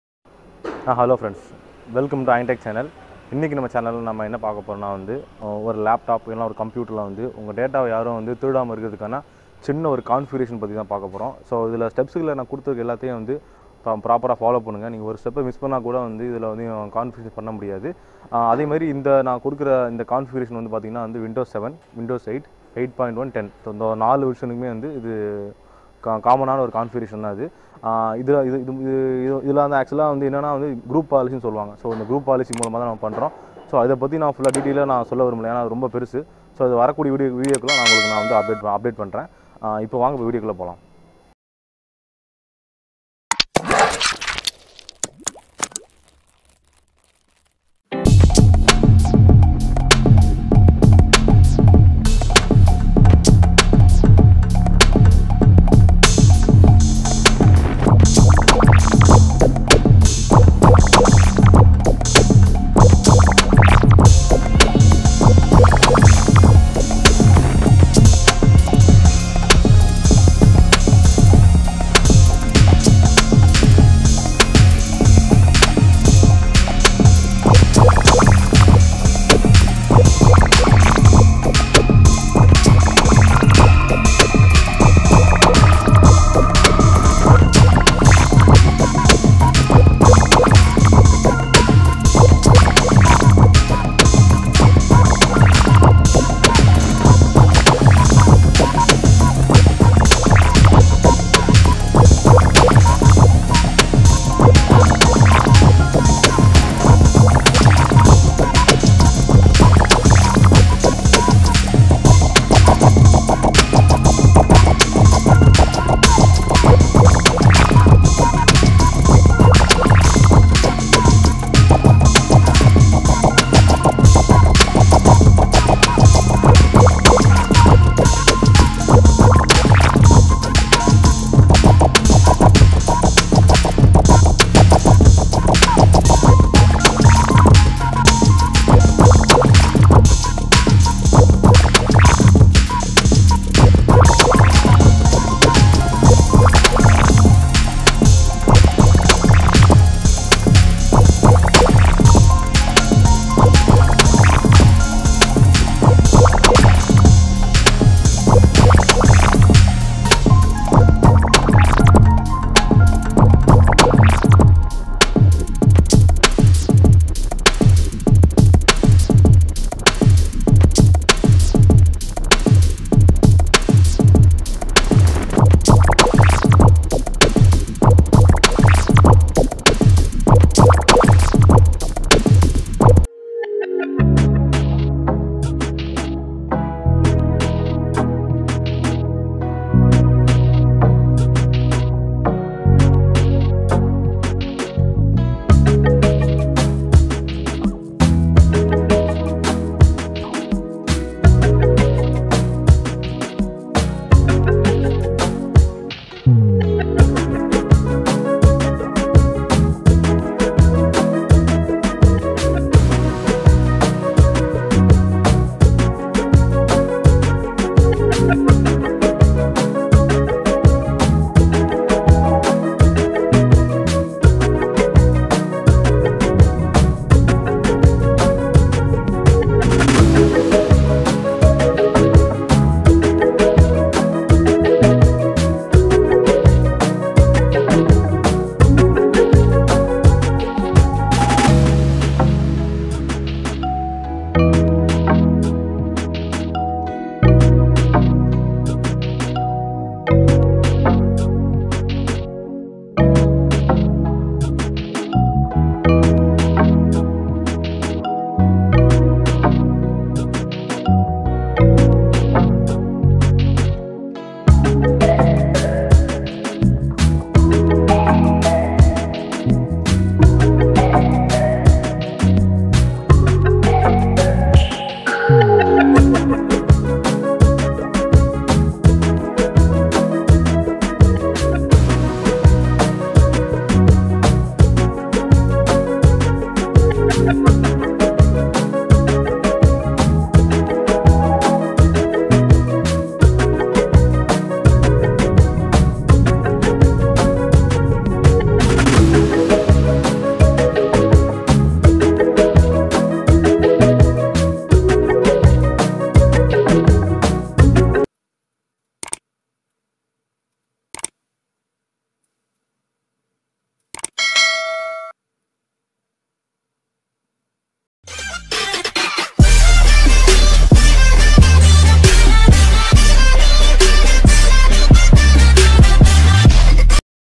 How to secure your personal data using GroupPolicy in tamil
In this video explained about the "How to secure your personal data using GroupPolicy in tamil"  given clearcut demo videos and
real time testing video uploaded kindly watch and learn , step by step explained about the "How to secure your personal data using GroupPolicy in tamil"

- - - - - - - - - - - - - - - Steps and Procedures- - - - - - - - - - - - - - -
1.   Goto the run option press the button {windows +R}
2.   Type gpedit.msc
3.   Click to computer configuration
4.   Expand the folder Administrative Templates
5.   Expand the folder Windows Components
6.   Expand the folder Remote Desktop Services
7.   Expand the folder Remote Desktop Session Host
8.   Click the folder Device and Resources Redirection
9.   In Right side page click the 5th policy
10. Click and Enable the 5th policy {Do not allow Clipboard redirection}
11. Click and Enable the 7th policy {Do not allow Drive redirection}
12. Goto the run option press the button {windows +R}
13. Type cmd - command prompt windows will open
14. In command prompt type;   gpupdate /force
15. After receive message policy updated successsfully
16. Restart your system
17. End
 - - - - - - - - - - - - - - -Iron Tech- - - - - - - - - - - - - - -

Channel Head : Mohamed Athikgur Rahuman M
Video Editor : Mohamed Athikgur Rahuman M
- - - - - - - - - - - - - - -Tamil version- - - - - - - - - - - - - - -
இந்த வீடியோவில்  "How to secure your personal data using GroupPolicy in tamil" தெளிவான விளக்கம் கொடுத்துள்ளேன் .
இந்த விடியோவின் பதிவுகளை தவறாமல் பார்க்கவும் , கொடுக்கப்படுள்ள அனைத்து விளக்கங்களும்
இன்றியமயாதவை படிபடியாக விளக்கங்கள் தெரிவித்துள்ளேன்.

- - - - - - - - - - - - - - -அயர்ன் Tech- - - - - - - - - - - - - -